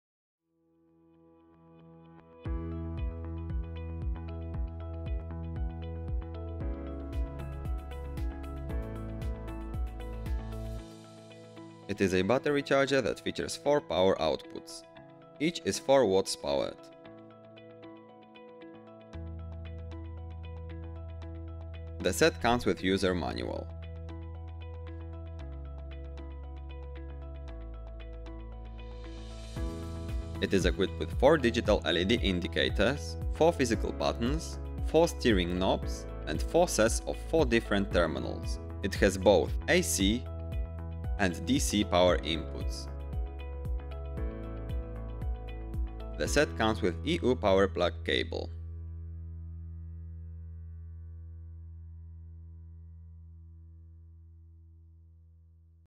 SKYRC SK-100079 - Charger: modelling - UNBOXING
Type of charger: modelling
TME Symbol: QUATTRO

Features:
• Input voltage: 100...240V
• Power: 4W
• Output current: 1A

Additional information:
• Gross weight: 481.9 g